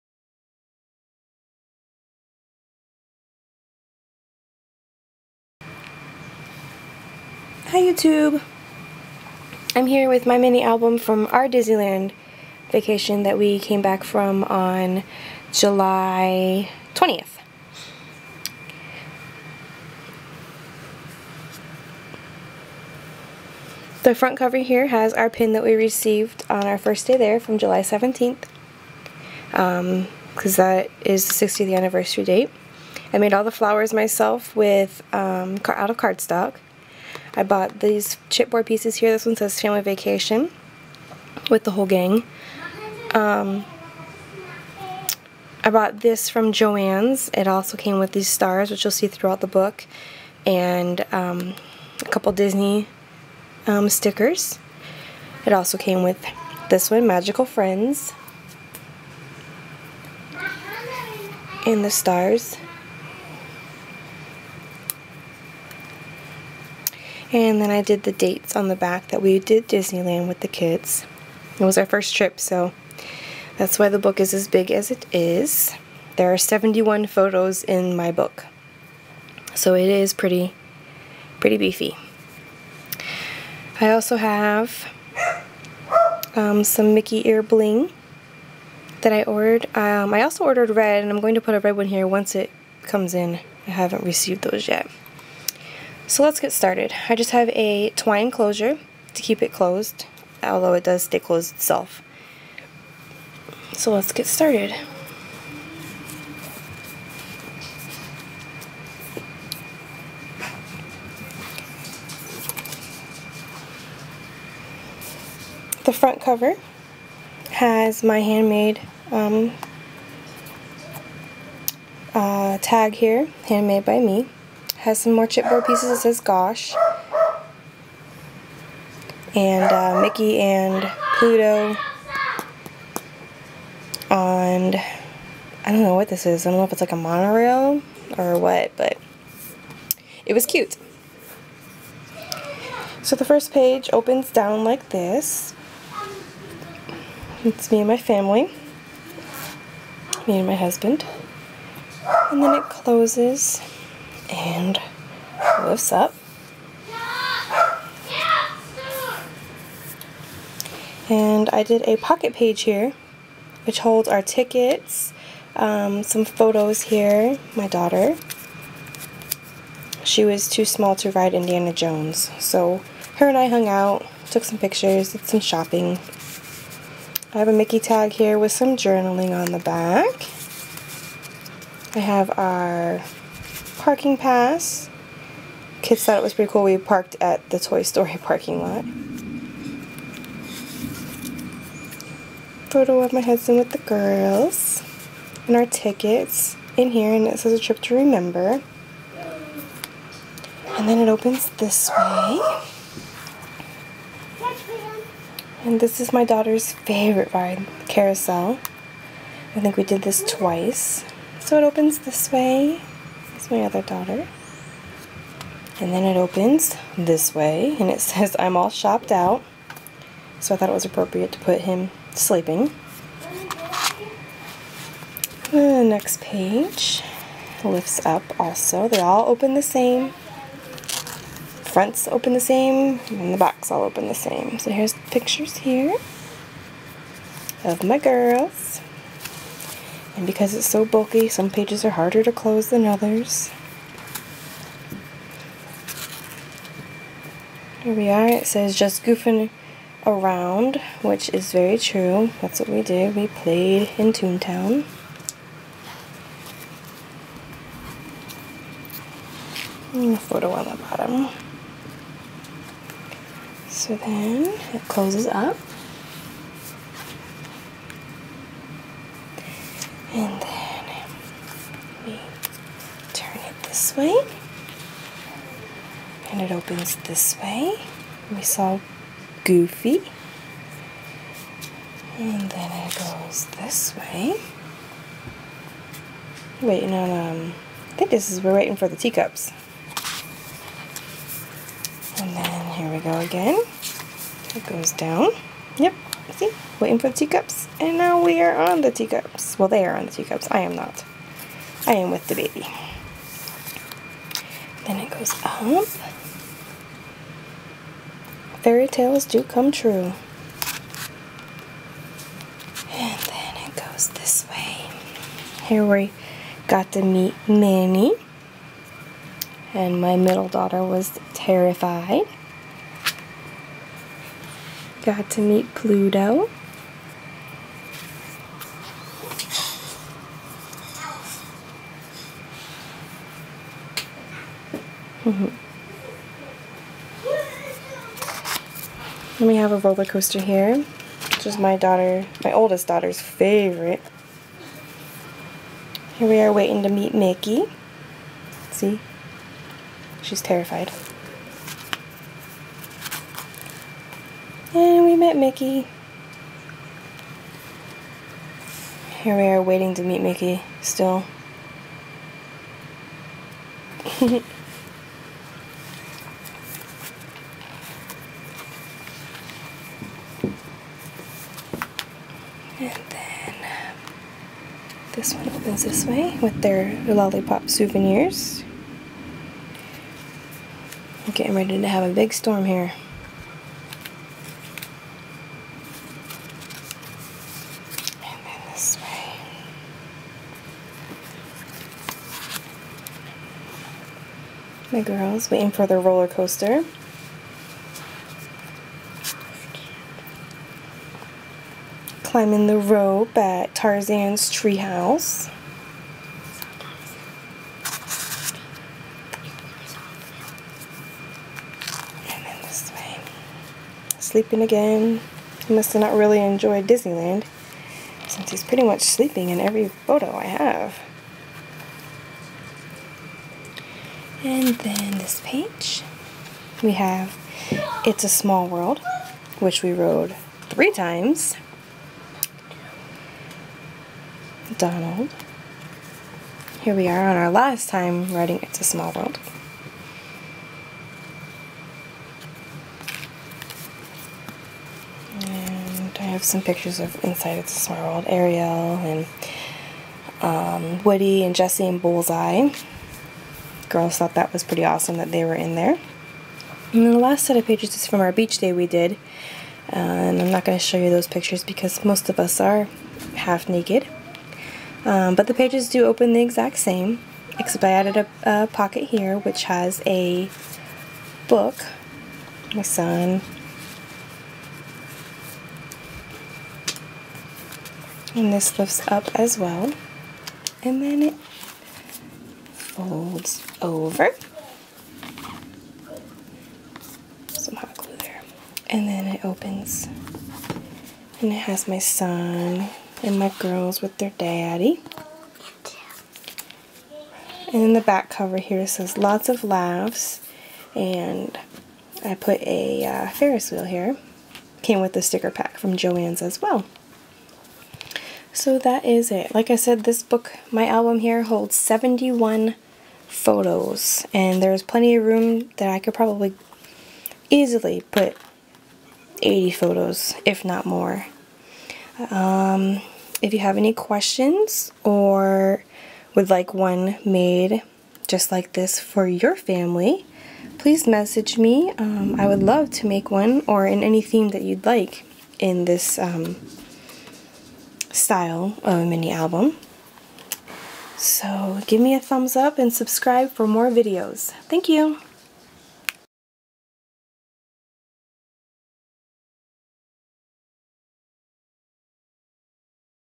Disney Mini Album - My Family
A mini album I made for my own family for our recent Disneyland trip. This album was so much fun to make.

I made all of the paper flowers on the front cover. Pages are made with cardstock. Chipboard cover!

Message me if you are interested in an album like this. I would love to create one in this style for you in any theme.